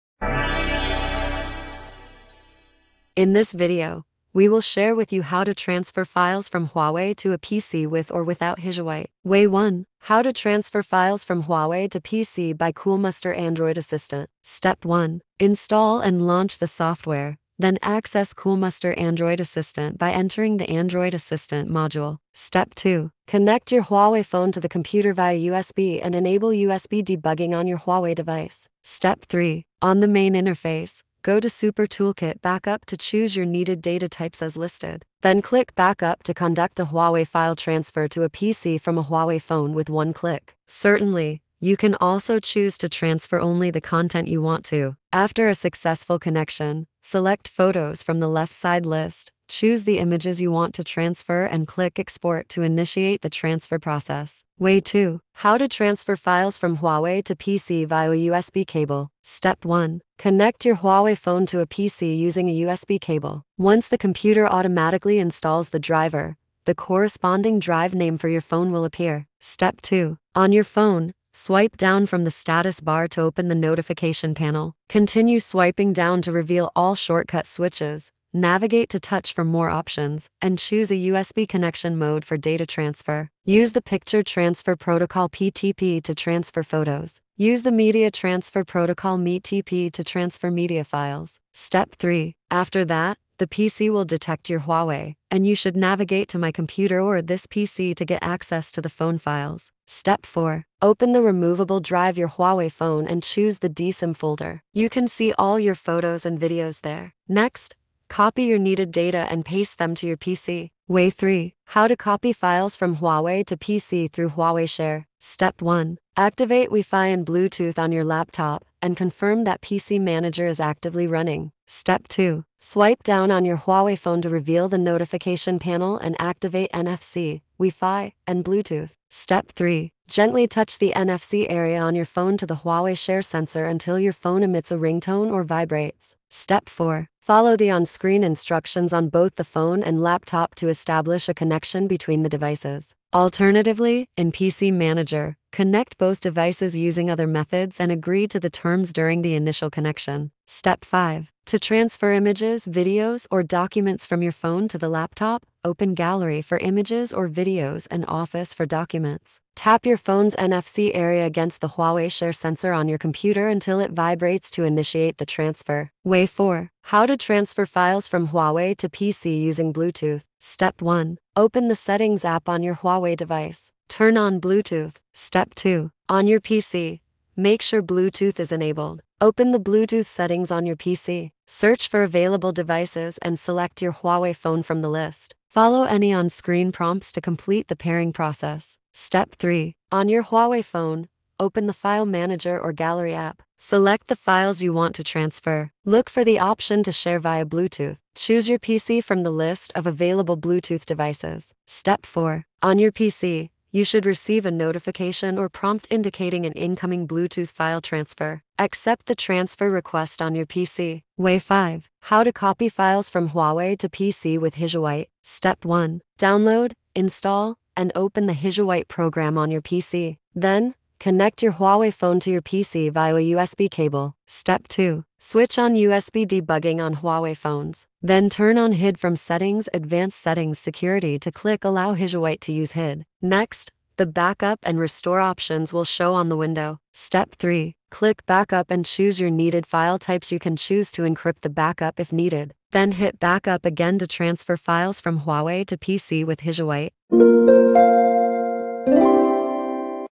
How to Transfer Files from Huawei to PC in 5 Ways
Can I transfer files from Huawei to a PC? Yes, of course. Please check this video to learn how to transfer files from Huawei to PC with or without Hisuite in 5 ways.

00:00 Start
00:09 Way 1. How to Transfer Files from Huawei to PC by Coolmuster Android Assistant
00:54 Way 2. How to Transfer Files from Huawei to PC via a USB Cable
01:50 Way 3. How to Copy Files from Huawei to PC Through Huawei Share
02:45 Way 4. How to Transfer Files from Huawei to PC Using Bluetooth
03:31 Way 5. How to Copy Files from Huawei to PC with Hisuite